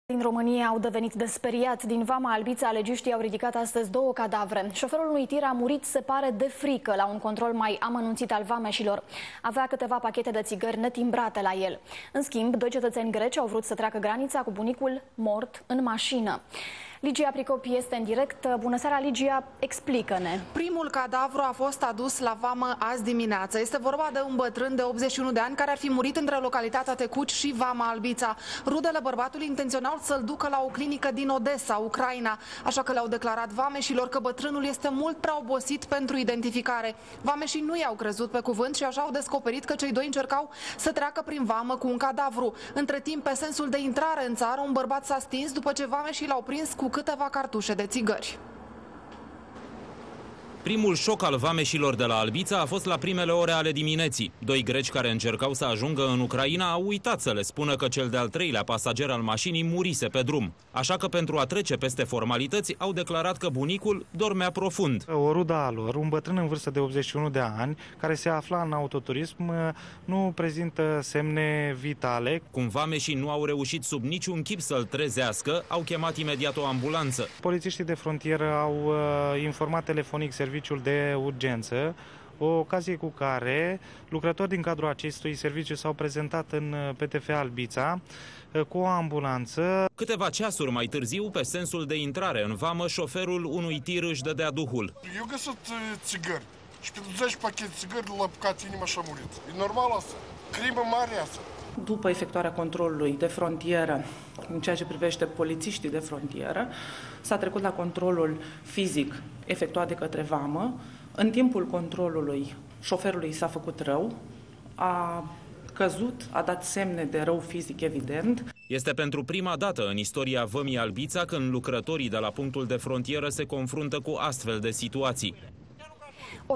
Un sofer a murit de frica la un control mai amanuntit al vamesilor 12 FEBRUARIE 2011
Vamile din Romania au devenit de speriat. Din Vama Albita legisti au ridicat azi doua cadavre. Soferul unui TIR a murit se pare de frica la un control mai amanuntit al vamesilor. Avea citeva pachete de tigari netimbrate la el. In schimb doi...